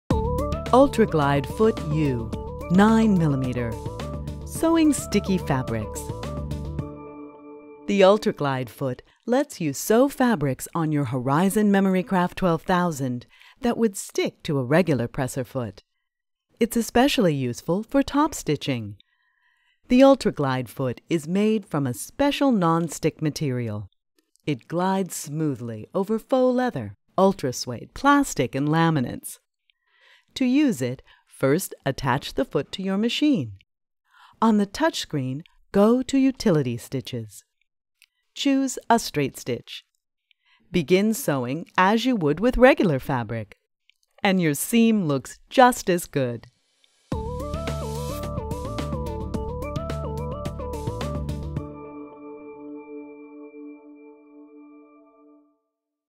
Ultra Glide Foot U
Ultra Glide Foot U is a 9MM foot that allows you to sew adhesive fabrics. The Ultra Glide Foot U is made of a non-stick material.